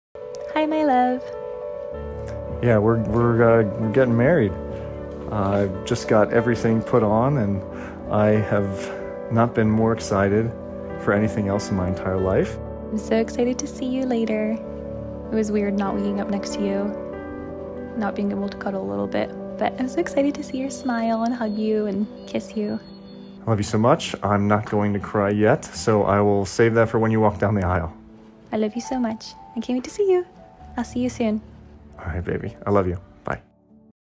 Leave a wedding day voicemail for your partner! 🤍 📞
What do you do if you don't want to see each other before your ceremony, but you'd still like to have a sentimental moment to look back on? Leave each other a wedding day voicemail or voice memo! After all, there's nothing quite like hearing your partner tell you how excited they are to marry you 🤍